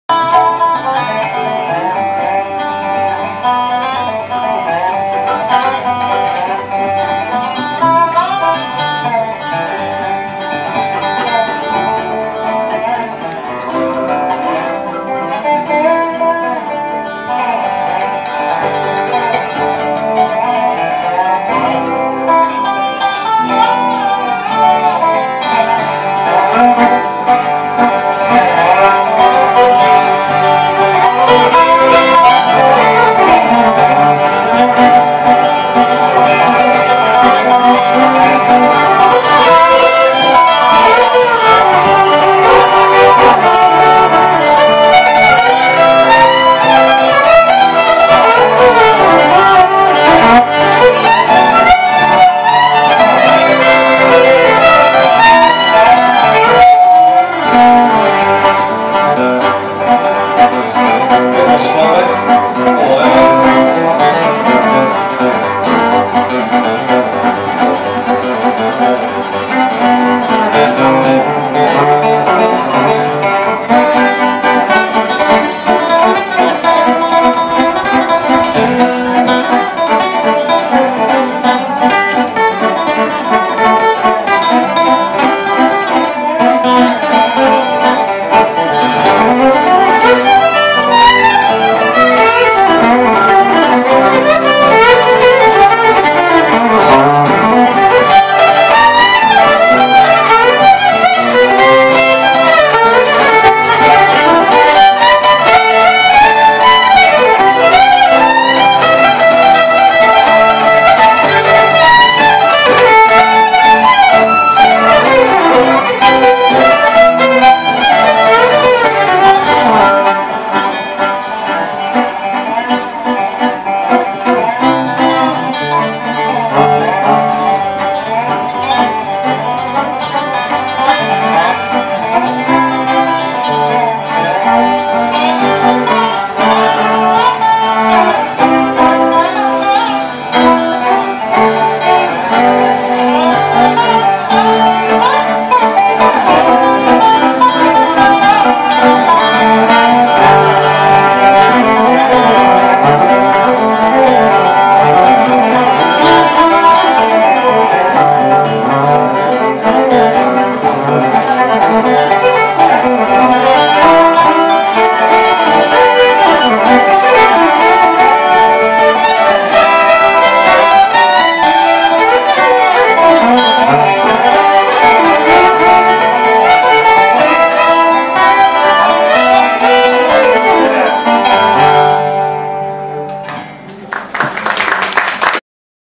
Pete Fidler, Charlie McCarthy & Andrew Clermont @ National Folk Festival, 2015.
Pete Fidler, Charlie McCarthy & Andrew Clermont jamming in the Pod @ National Folk Festival, Canberra, 2015.